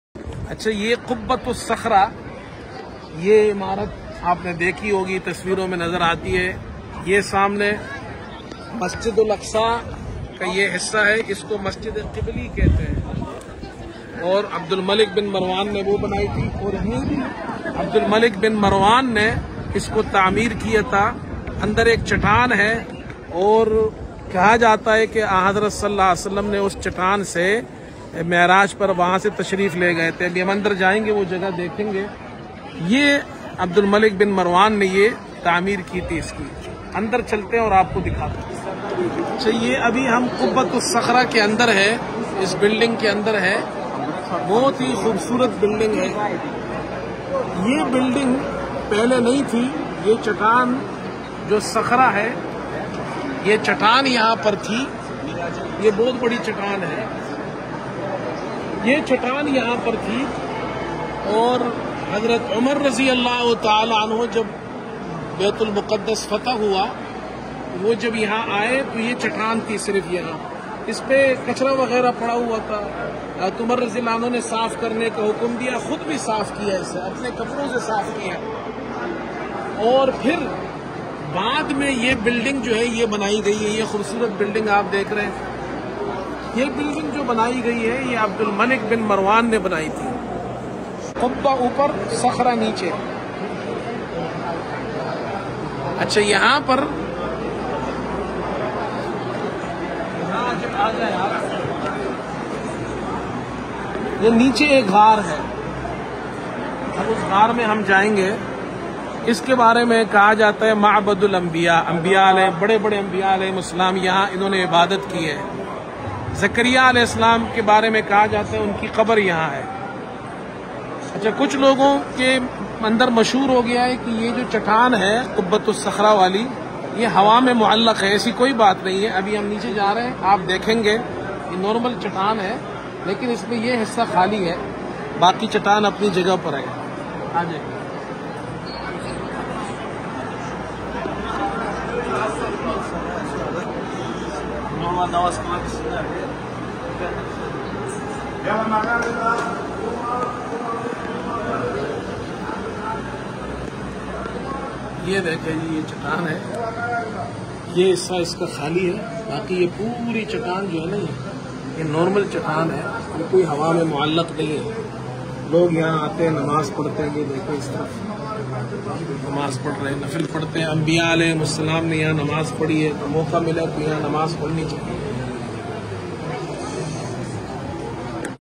قبۃ الصخرۃ ۔۔۔ وہ مقام جہاں سے رسول اللہ ﷺ معراج پر تشریف لے گئے ۔۔۔masjidaqsa
قبۃ الصخرۃ ۔۔۔ وہ مقام جہاں سے رسول اللہ ﷺ معراج پر تشریف لے گئے ۔۔۔ انبیاء علیھم السلام کی عبادت کی جگہ.